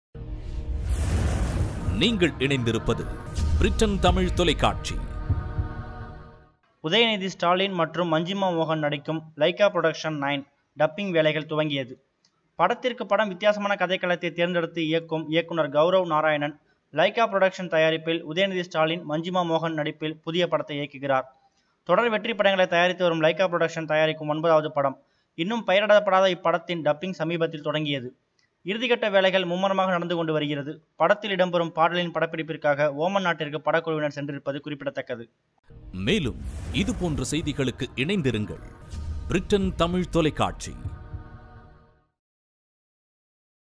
Udayanidhi Stalin - Manjima Mohan in new film
Udayanidhi Stalin - Manjima Mohan in new film - An unnamed movie produced by Lyca productions has reached the stage of dupping.